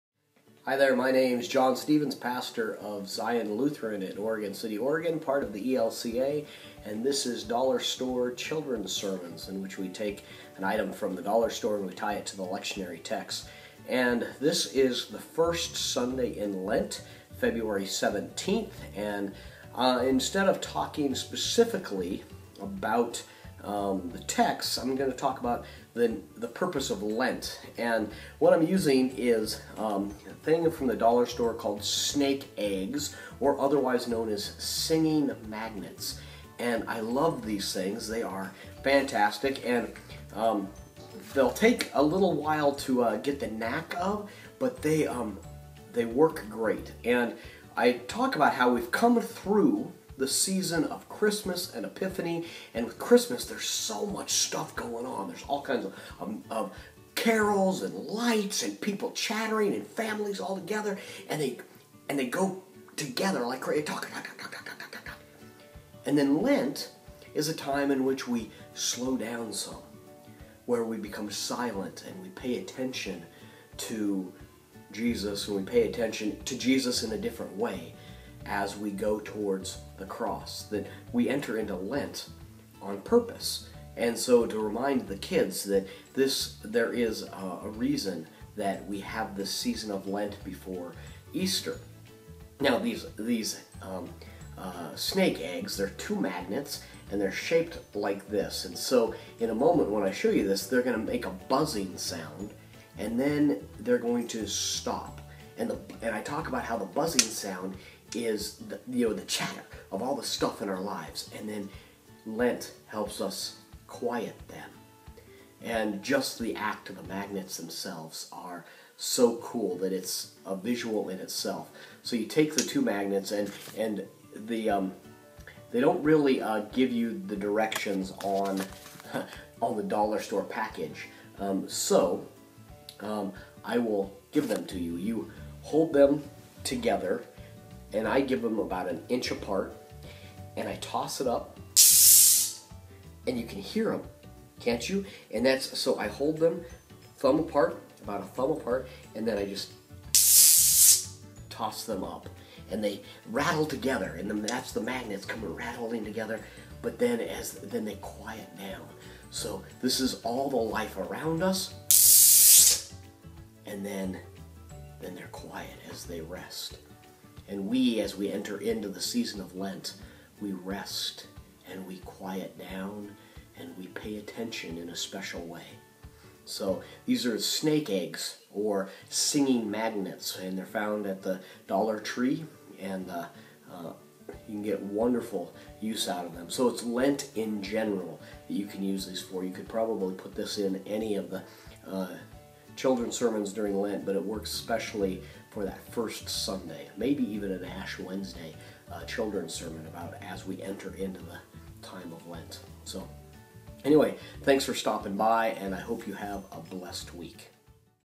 Dollar Store Children Sermons - Snake Eggs
Using singing magnets/snake eggs, I talk about entering into Lent, as a time to be quiet and to pay attention.